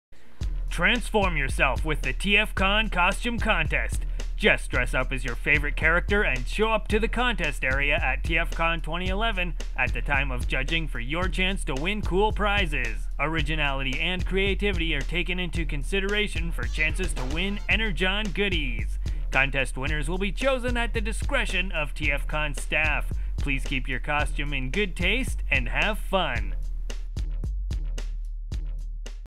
Transform Yourself: TFcon 2011 Costume Contest
Transform Yourself, the TFcon 2011 Costume Contest returns to Canada's 10th annual Transformers Convention!